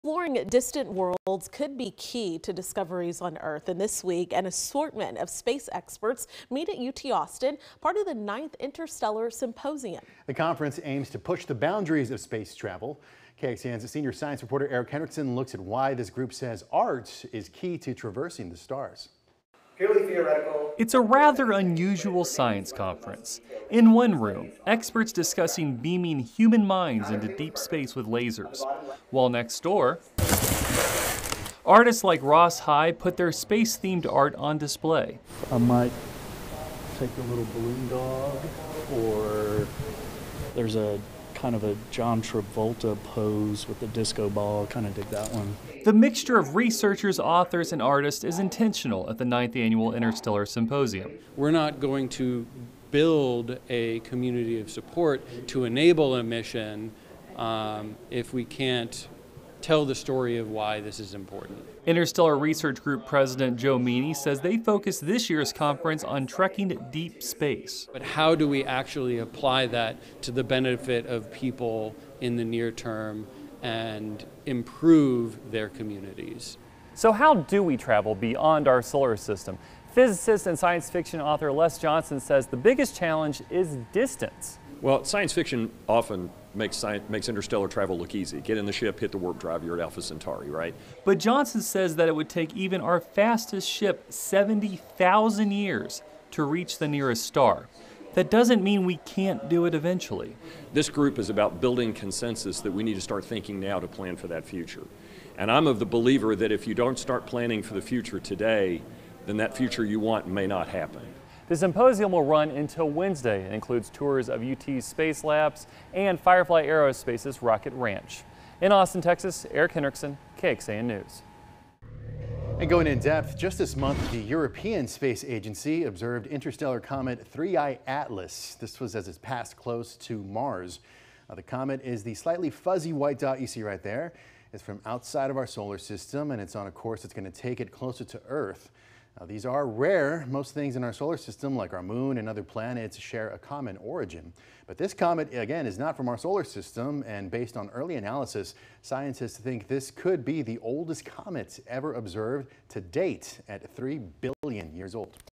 Interstellar travel explored at conference hosted by UT Austin
Exploring beyond our solar system could have lasting impacts here on Earth, and scientists, creatives and industry experts gather at the University of Texas at Austin this week to discuss the possibilities.